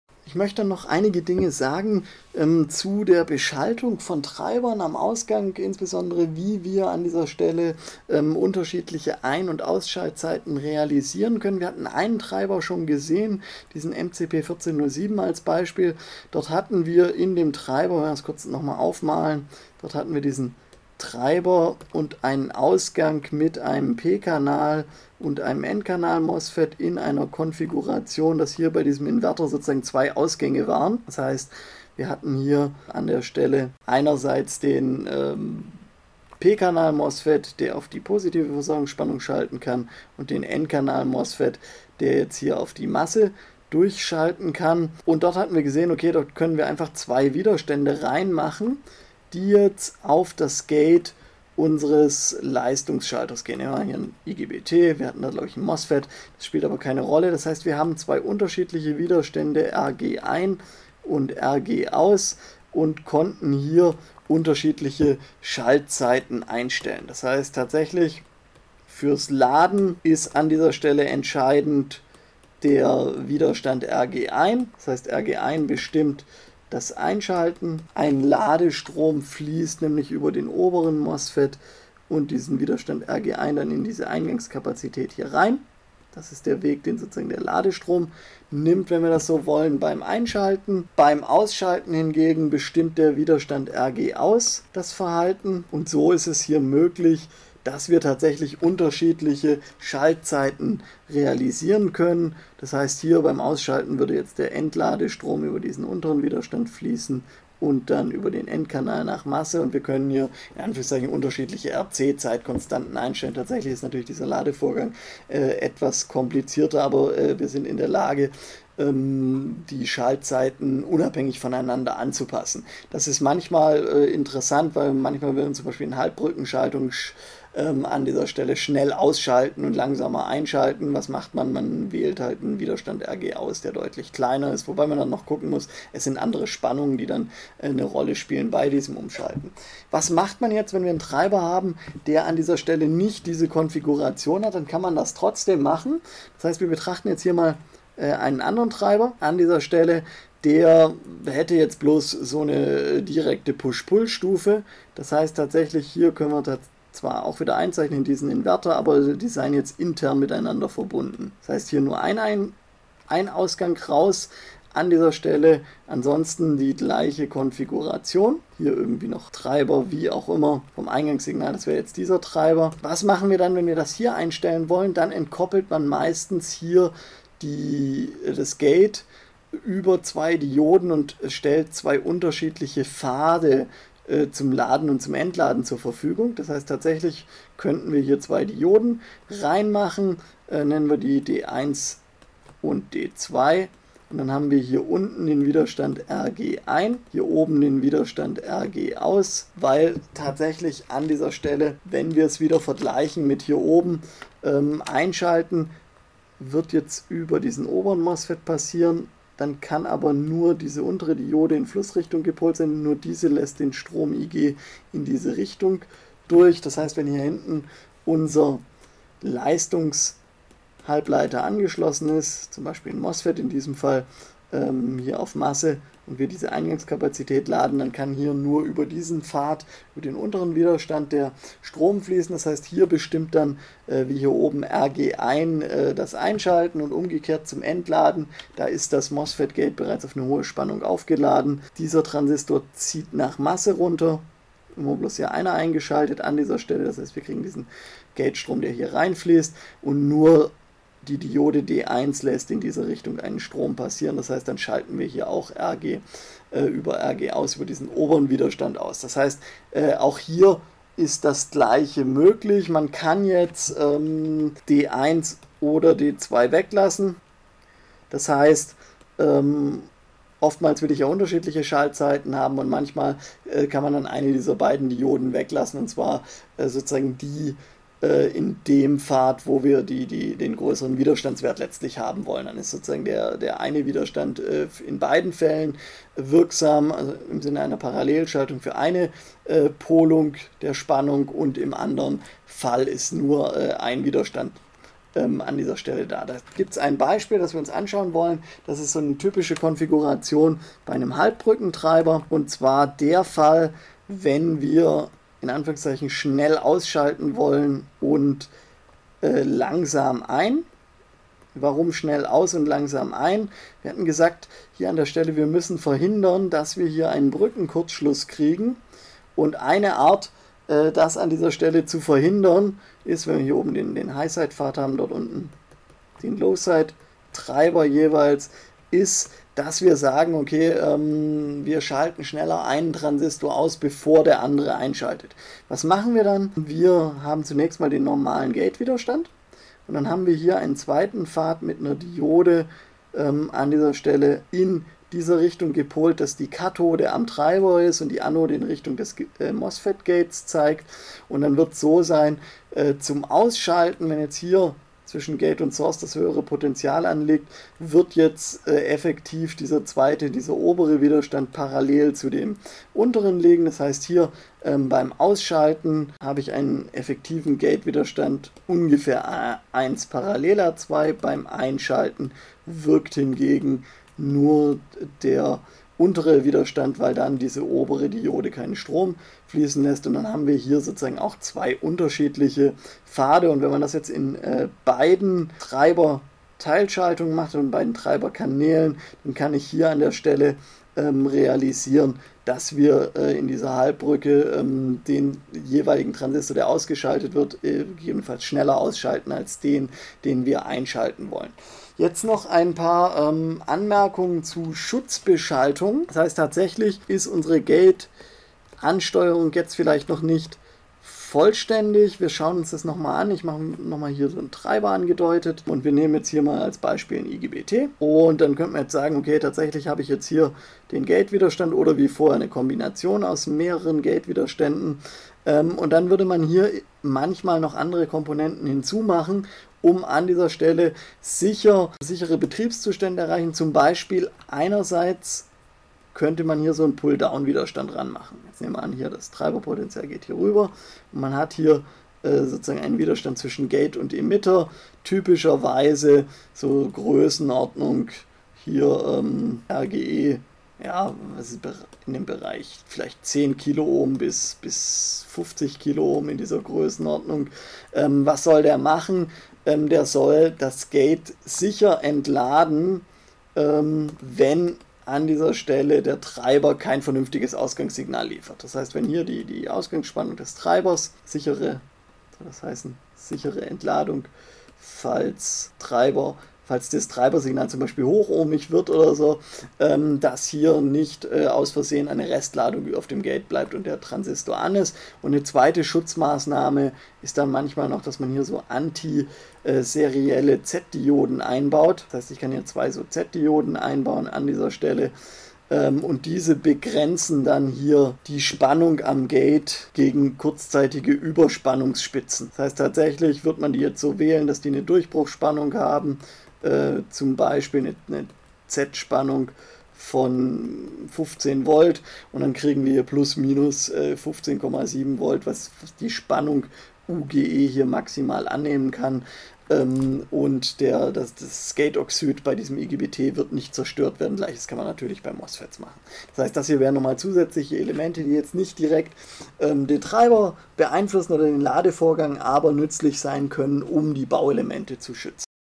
LE3_6g Beschaltung von Gate Treibern für Mosfet/IGBT
Vorlesung Leistungselektronik im Studiengang Mechatronik an der DHBW Stuttgart Campus Horb.
In diesem Teil wird besprochen, wie unterschiedliche Schaltzeiten für das Ein- und Ausschalten bei integrierten Gate Treibern erreicht werden kann und zudem manchmal nötige zusätzliche Schutzschaltungen vorgestellt